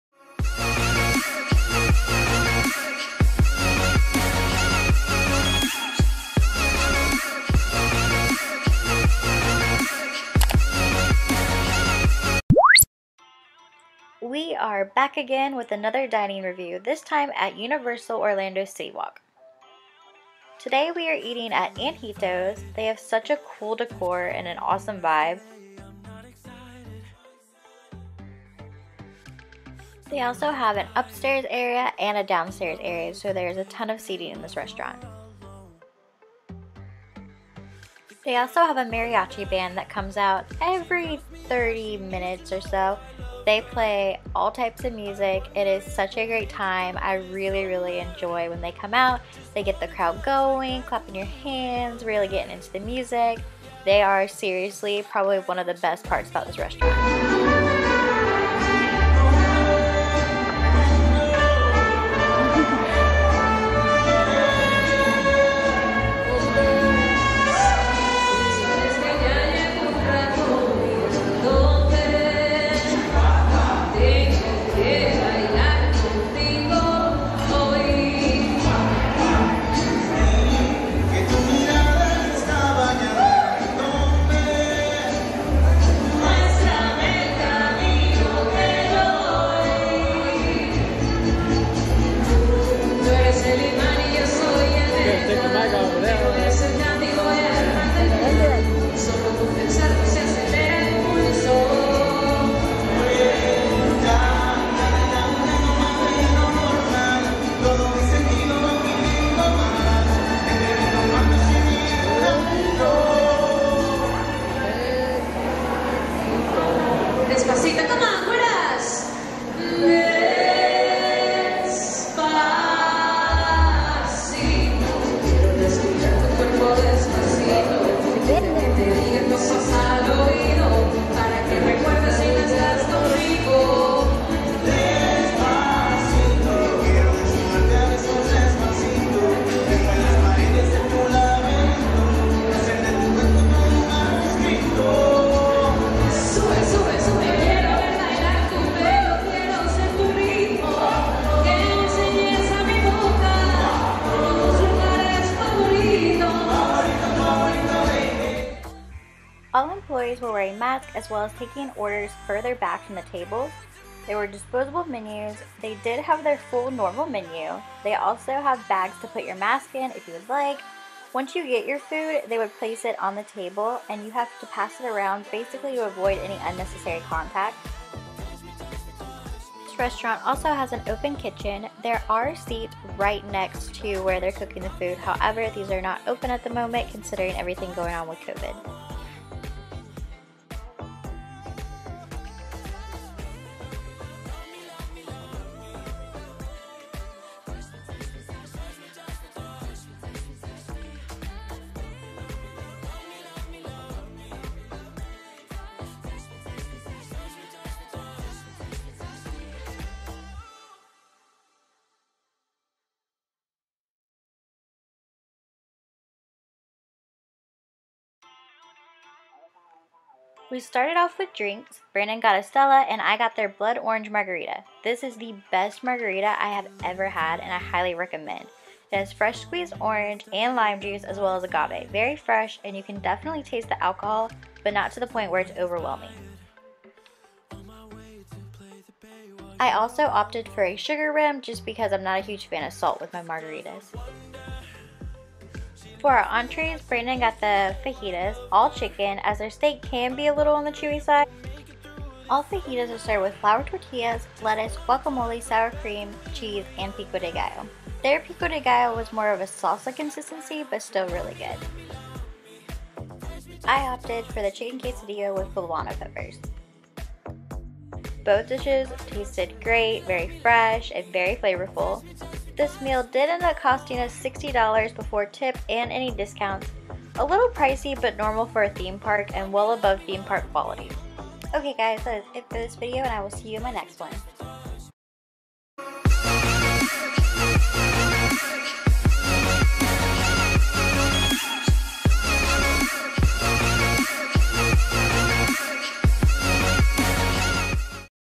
Antojitos Dining Review | City Walk Universal Orlando
We are back at it again with the dining reviews! This time at Universal Orlando's City Walk. We got to go to Antojitos a Mexican restaurant located up the hill on the left in City Walk. A little tucked away but such an amazing restaurant.
equipment I use
cannon m50

them! :)

Intro by SIMPLY INTROS!